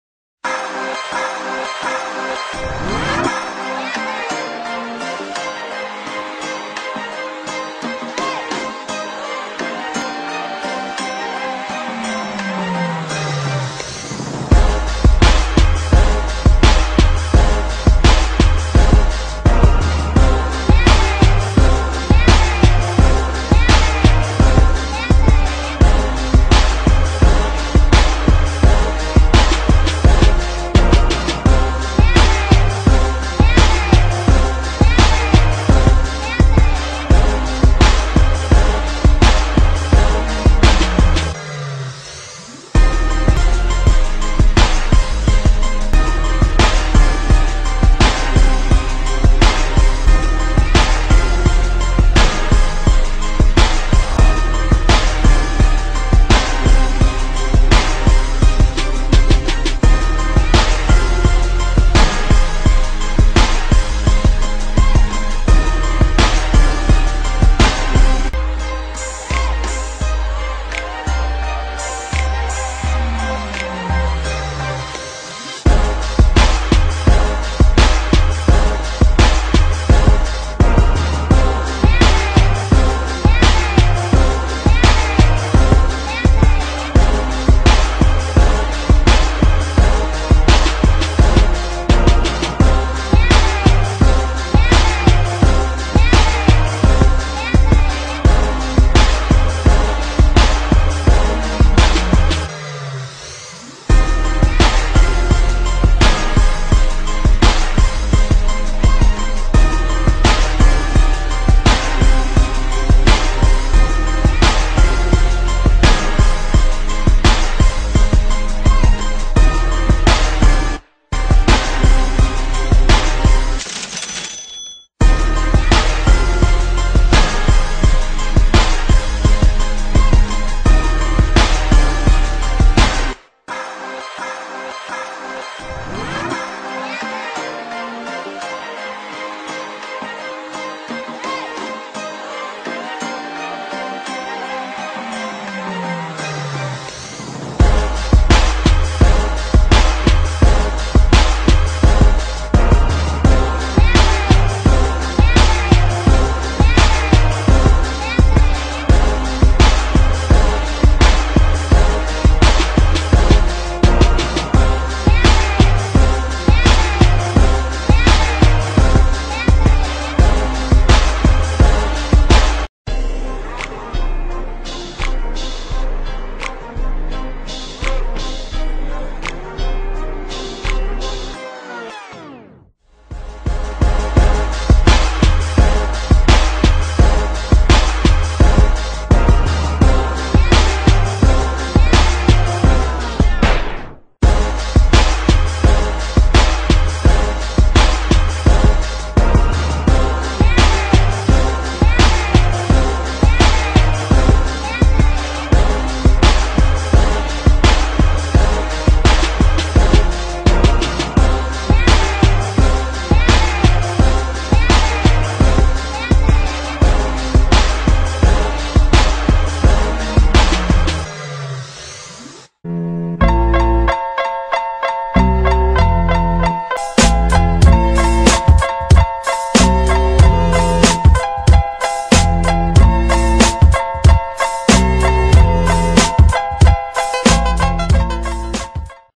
FSU Running Backs Preview
Getting Ready for 2009!  Get hyped for our running backs!  We have great depth in Jermaine Thomas, Ty Jones, and Tavares Pressley.  Aside from the abysmal outing at the FSU Spring Game(5 fumbles), expect great things from these young running backs.